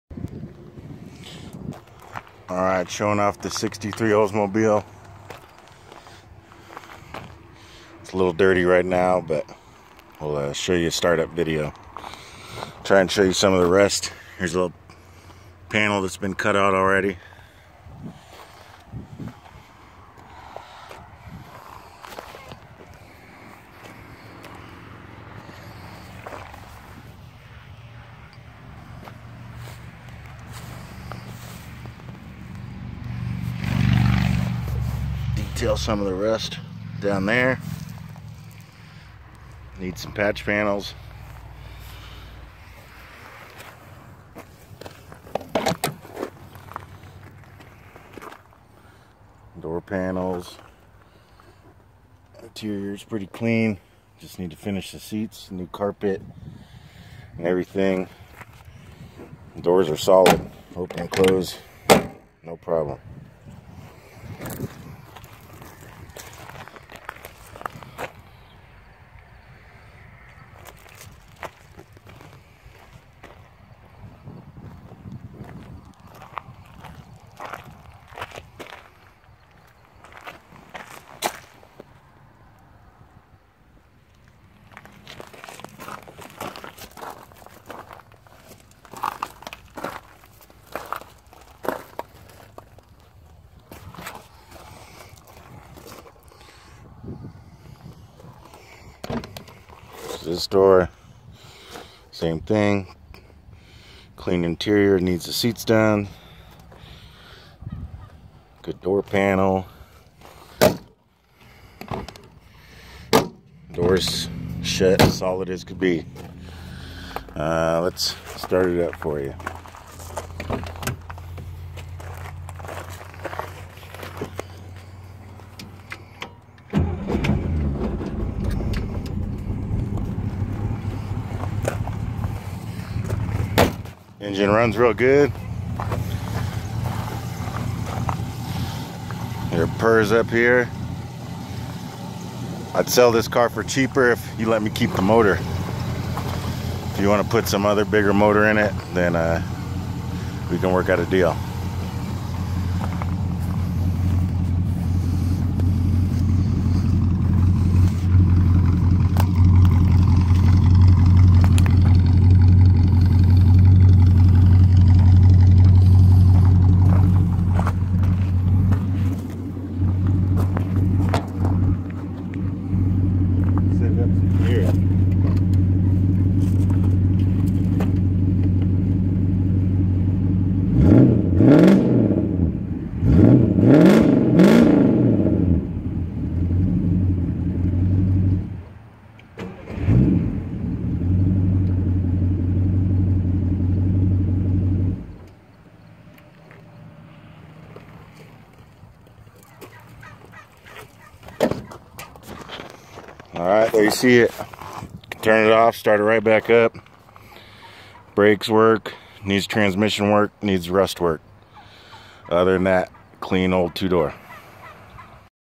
1963 Oldsmobile F85 cutlass, 215, start and walk around
215 aluminum V8, 4 barrel  glasspacks, for sale in Palisade, CO